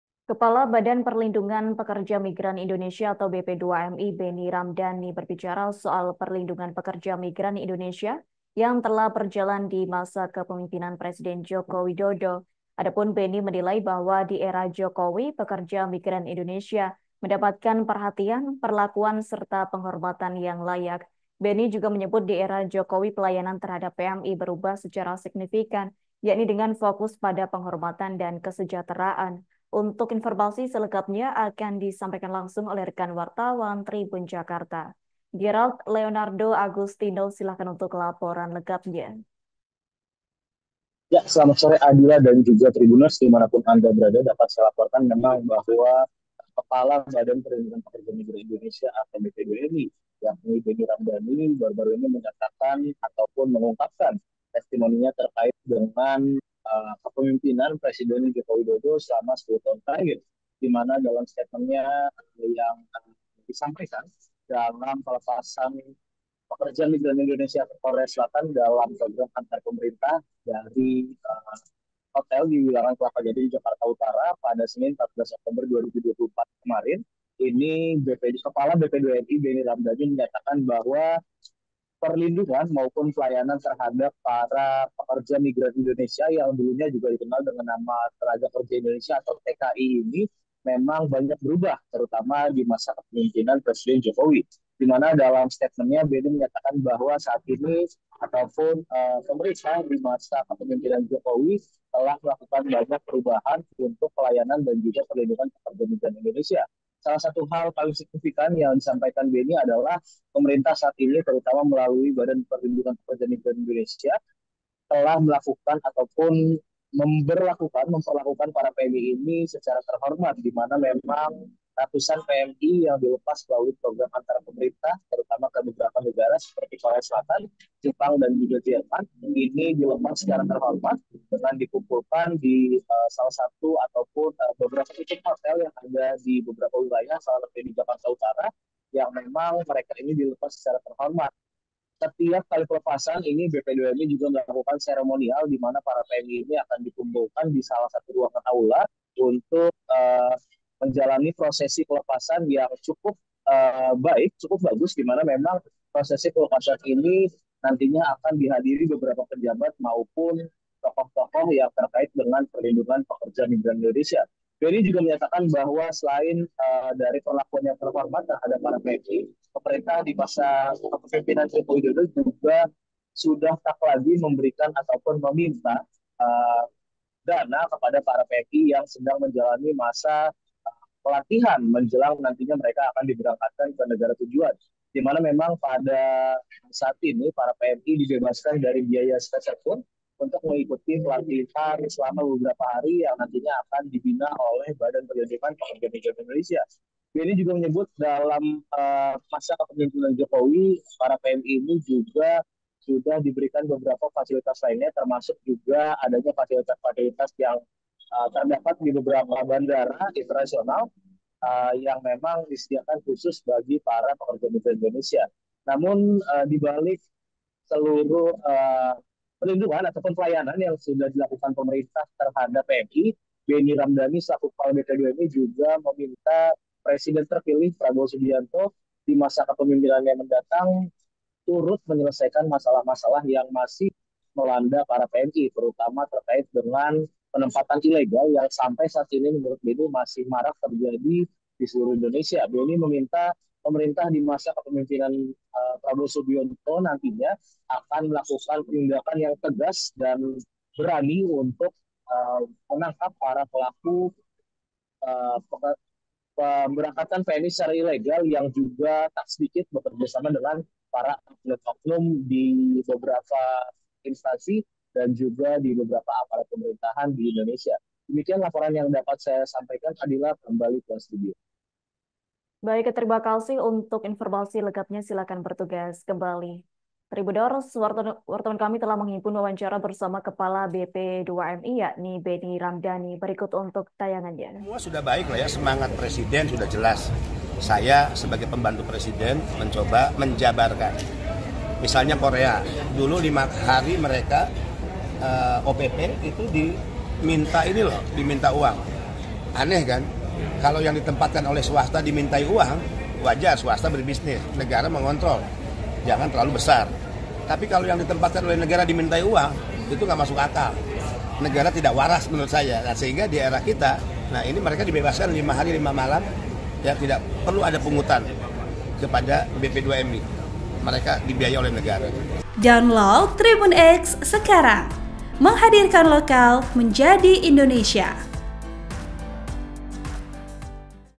Kepala BP2MI Puji Era Jokowi: Pekerja Migran Diperlakukan Terhormat, Prabowo Pikul Tantangan Besar
Laporan wartawan, Tribun Jakarta - Gerald Leonardo Agustino
TRIBUN-VIDEO.COM - Kepala Badan Perlindungan Pekerja Migran Indonesia (BP2MI) Benny Rhamdani berbicara soal perlindungan pekerja migran Indonesia yang telah berjalan di masa kepemimpinan Presiden Joko Widodo.

Benny menilai, di era Jokowi pekerja migran Indonesia mendapatkan perhatian, perlakuan, serta penghormatan yang layak.

Benny menyebut, di era Jokowi pelayanan terhadap PMI berubah secara signifikan, dengan fokus pada penghormatan dan kesejahteraan. (*)

Program: Live Update
Host: Adila Ulfa Muna Risna
Editor Video: Ika Vidya Lestari
Uploader: Ramadhan Aji Prakoso